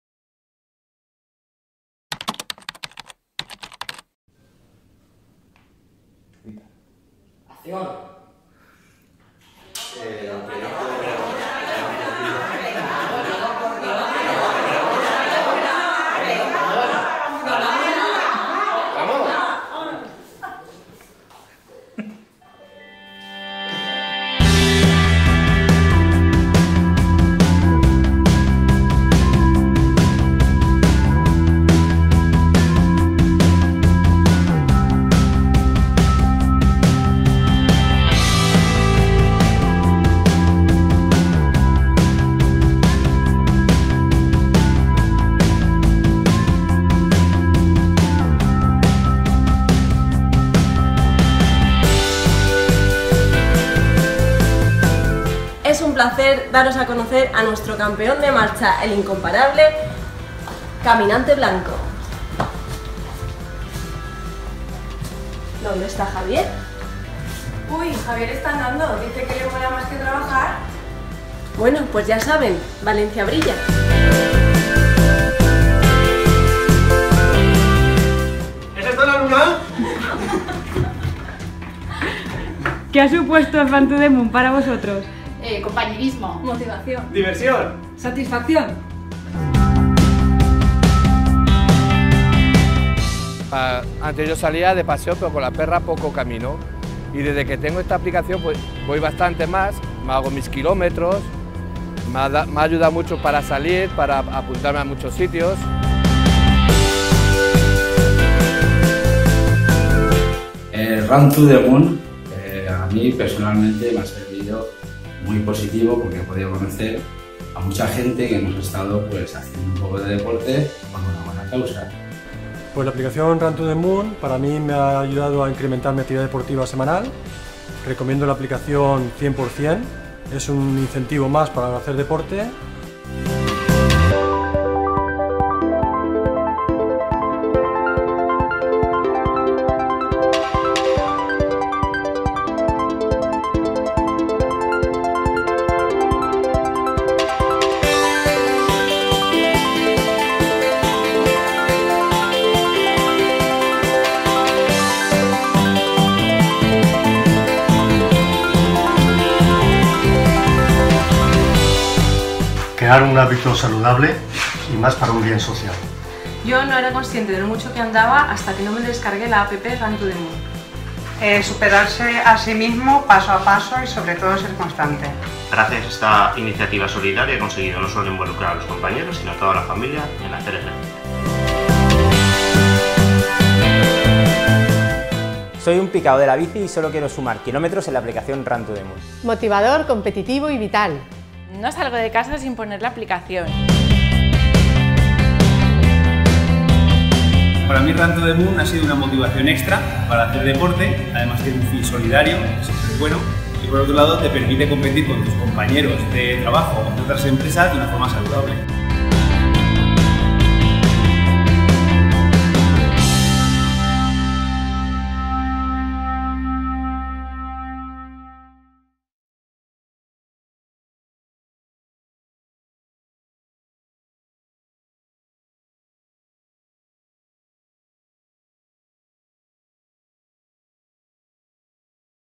Valencia Brilla - Run to the moon
Estas empresas se han unido en el objetivo de“Conquistar la Luna”. Recorrer entre todos los participantes de las diferentes organizaciones la distancia que separa la Tierra de la Luna a través de la APP móvil “Run to the Moon”.

Andar, correr, ir en bicicleta, patines e, incluso, en silla de ruedas.
 Todo contabilizará para conseguir el objetivo final que se tangibilizará en una donación a los Premios Hospital Optimista